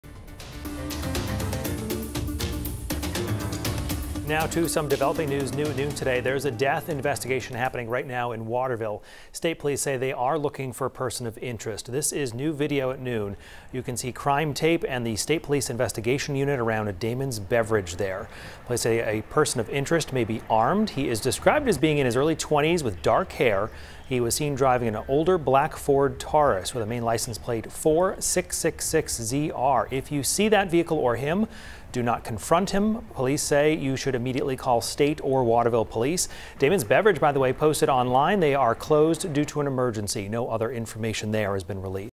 Police seek person of interest in Waterville death investigation
The man may be armed, Maine Department of Public Safety spokesperson Shannon Moss said.